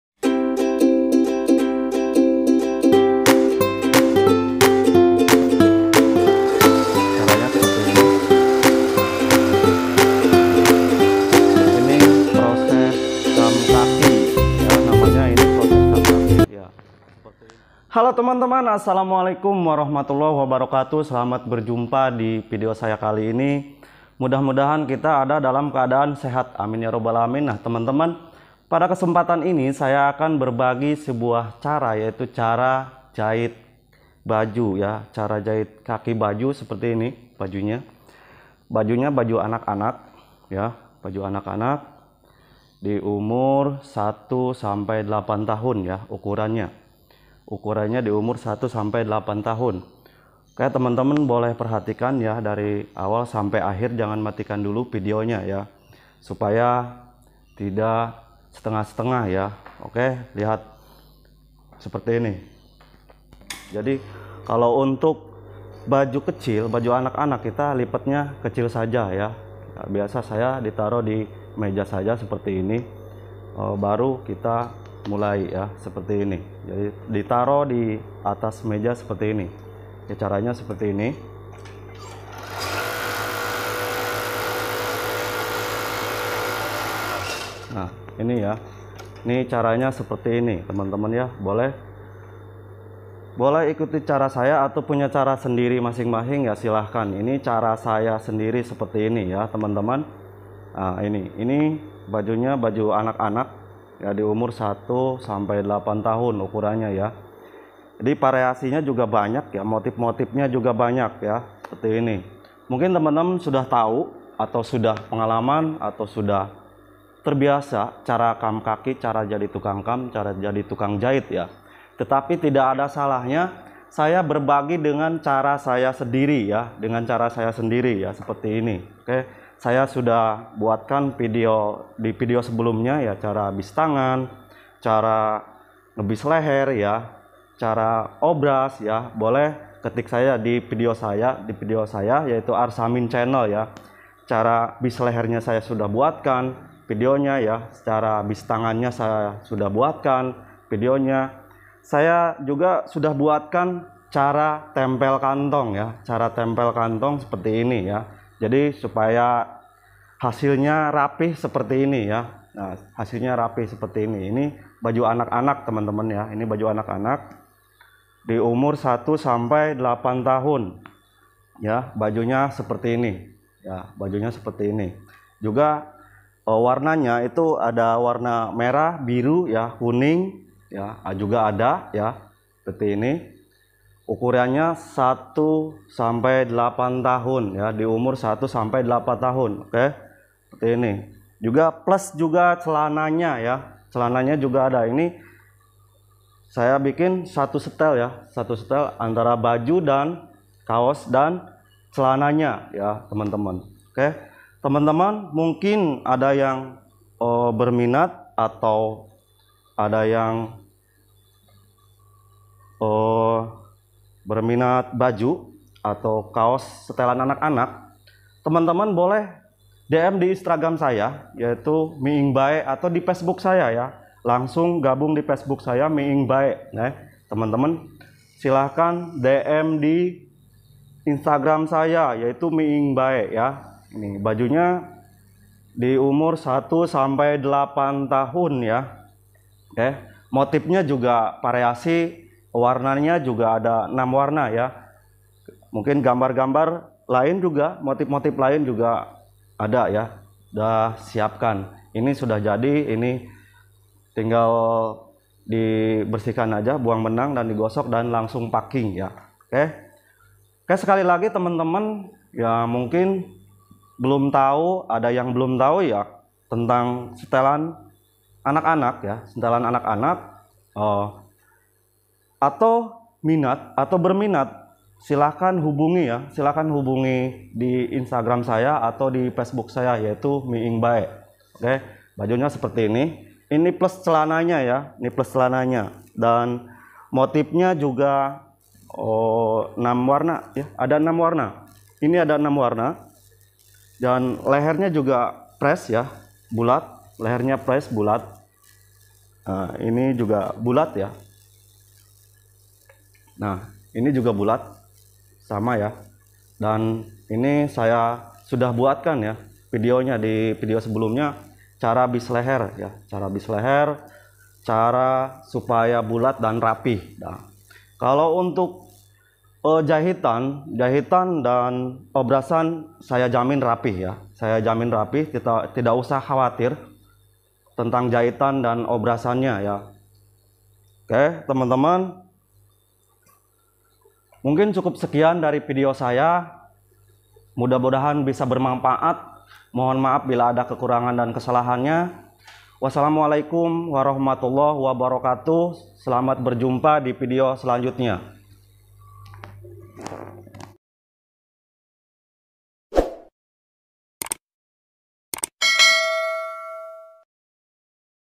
cara cepat menjahit baju
cara cepat menjahit baju kompeksi Jakarta kususus kaos stelan anak anak menggunakan mesin jahit  mesin Kam dan mesin obras dengan cara sendiri mohon maap bila ada kekurangan di Vidio ini semoga bermanpaat  sascreb ya

bila teman2 berminat dengan stelan kaos anak silahkan DM Intagram , miingbae